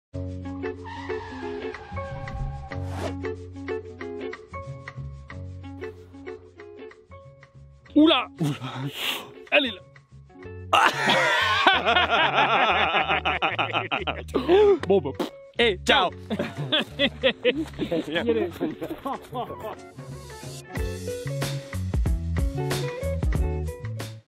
CIAO - Cara Compagnie (Mini-Teaser) - 2018 / Metlili.net
Raphaël Caputo (Réalisateur, Cadreur, Monteur, Colorimétrie)

Vidéo réalisée par Cordialement Bisous du collectif Balézocirque

Artistes : Colas Rouanet et Mattia Furlan

Filmé au Festival de l'Echo du Caquetoire à Cheverny (41) en août 2018.
Ce duo déjanté produit une rythmique qui nous emporte à partir de la jonglerie rebond dans toutes les parties de la Caravane.